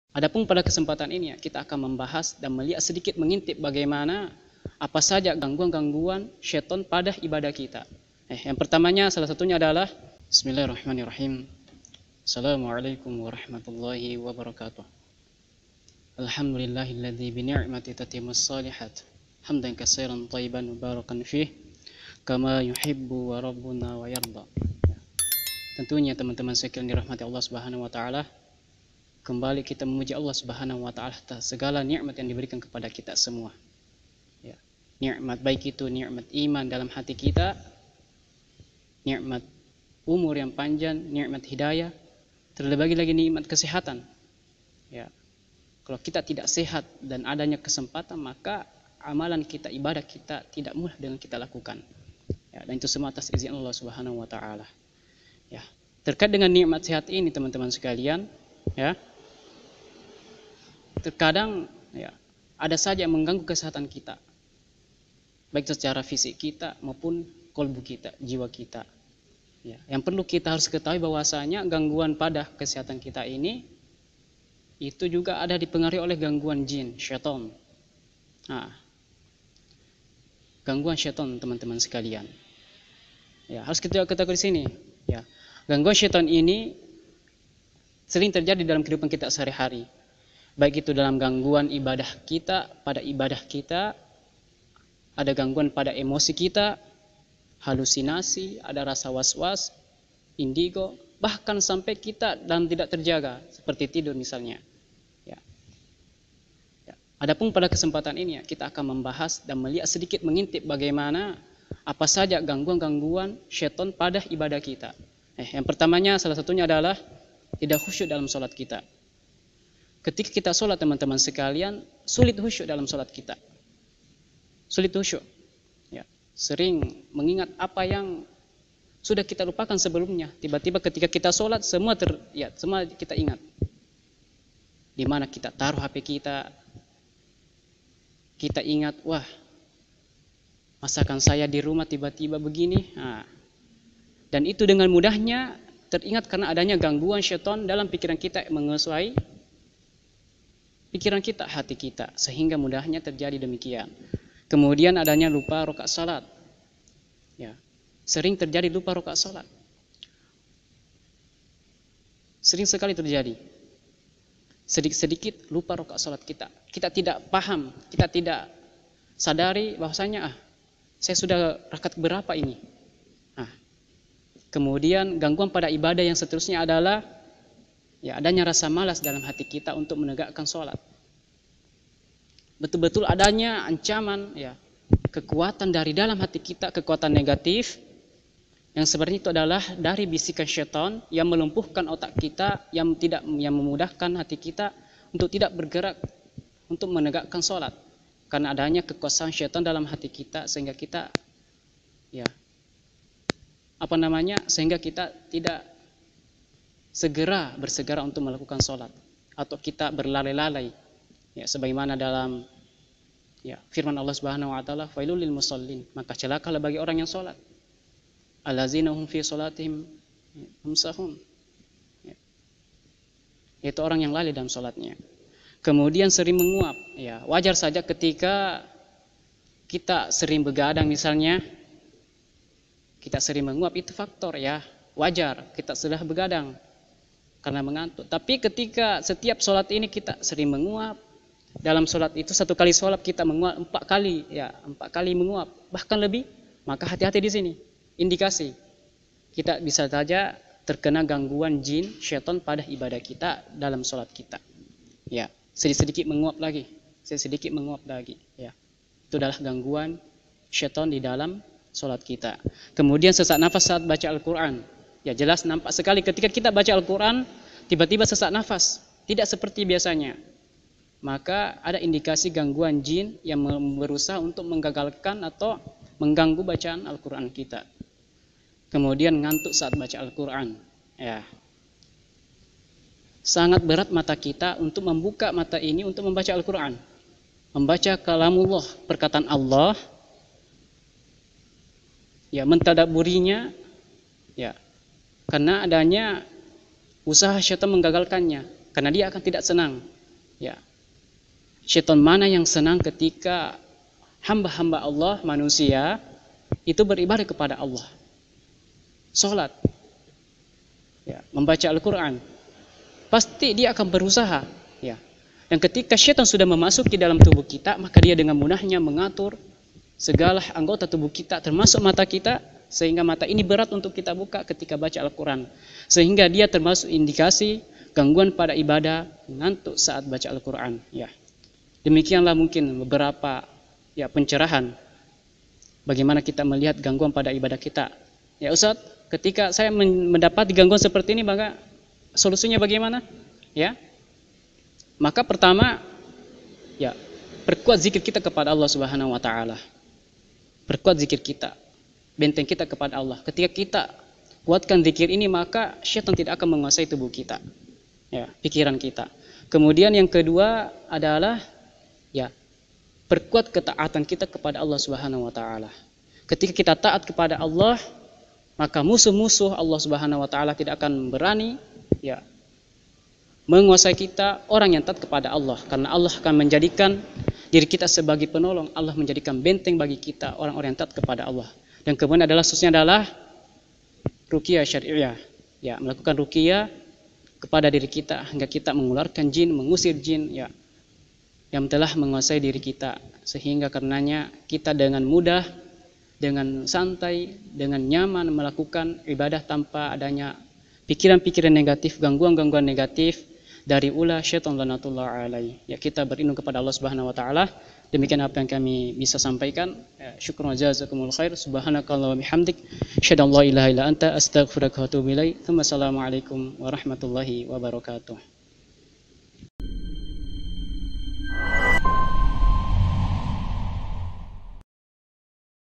GANGGUAN JIN SAAT IBADAH!
*GANGGUAN JIN PADA IBADAH*

🎙️Ust. Irfan Anwar _hafidzhahullah_ ▪️(️Ketua Div.Ruqyah & bendahara rehab hati sinjai / Imam Masjid Miftahul Khair)

⬆️⬇️ ⏰ ± 06:55

✳️ Jika dirasa bermanfaat, disilahkan share Video singkat ini...
Semoga anda ikut mendapatkan pahala jariyah.